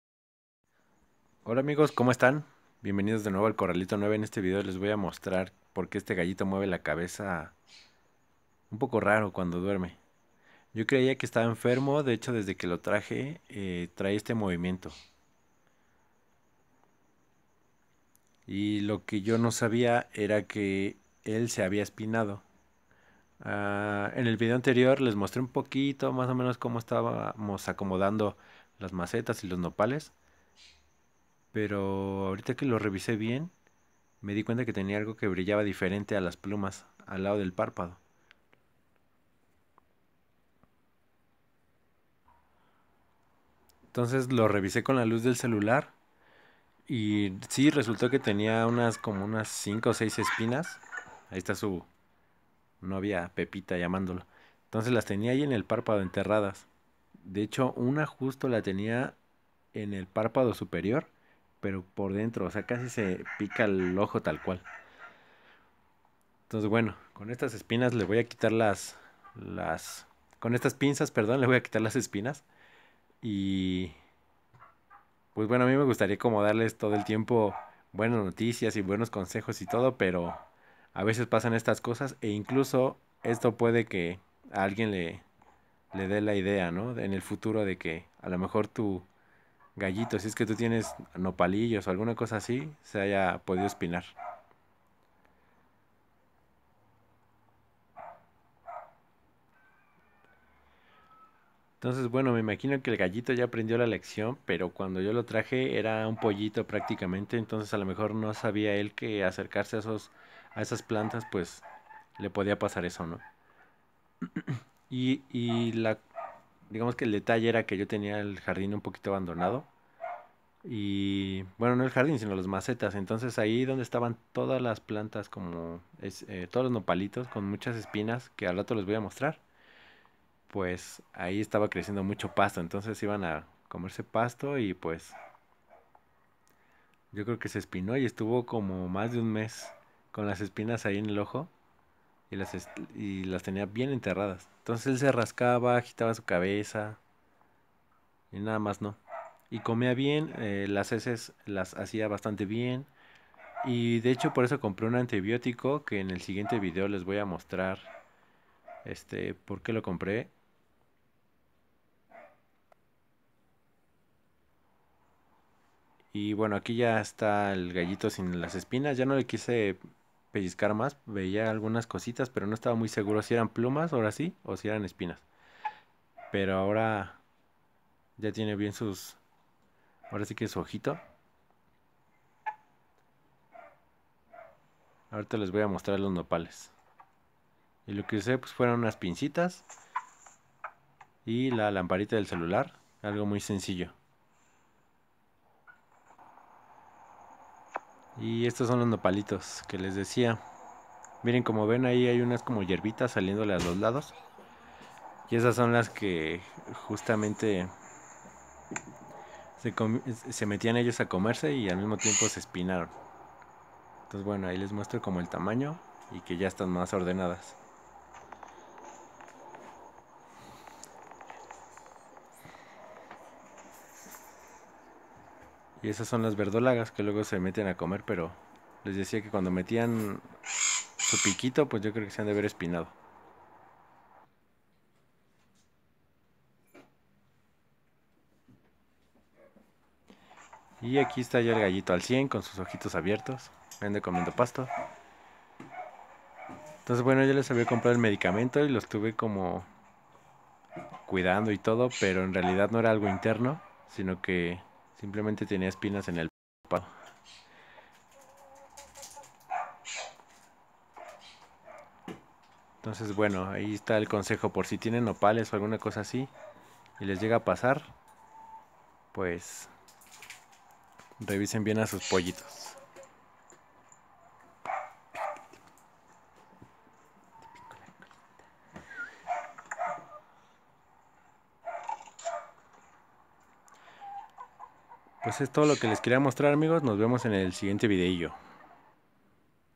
Pollito enfermo agitando la cabeza | cuidado con las espinas 😟🌵 | Ep. 26
Hola amigos!!!
Por fin se curo mi pollito Sebright, tenia unas espinas en los ojos, que le causaban malestar y la molestia no lo dejaba ni dormir ni comer, tengan cuidado si es que tienen nopales.
Un descuido mio que por fin se soluciono.